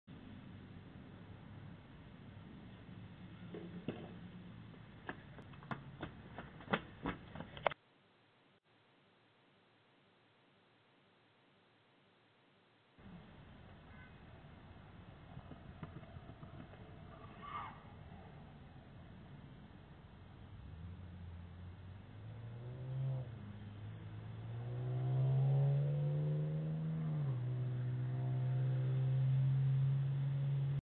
Goodenow Trespassing and Harassing My Family
This is the Goodenow child who flipped off my camera in my driveway on May 2nd and has trespassed before. He has also harassed my daughter at school.